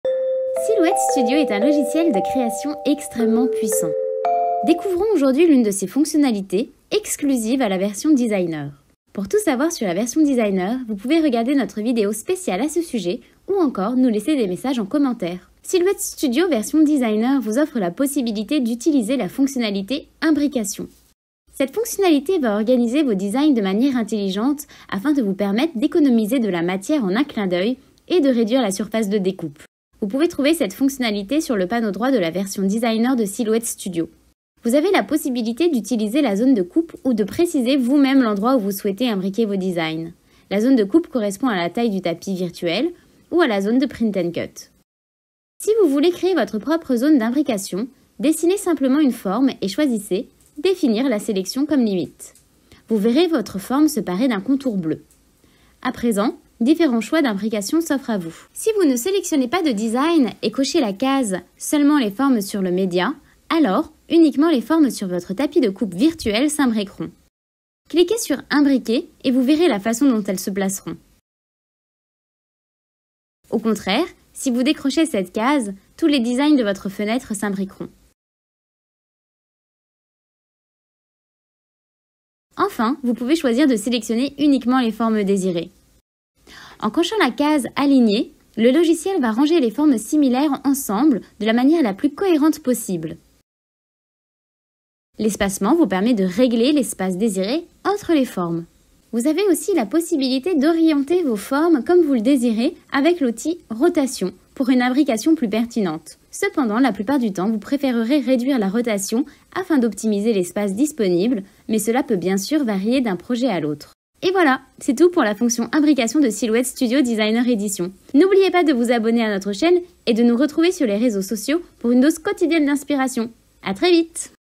Comment utiliser la fonction imbrication de Silhouette Studio Designer ? tuto Portrait Cameo
💙Vous cherchez une machine de découpe ? Vous avez besoin d'une lame, d'un tapis, de vinyle ou de flex ?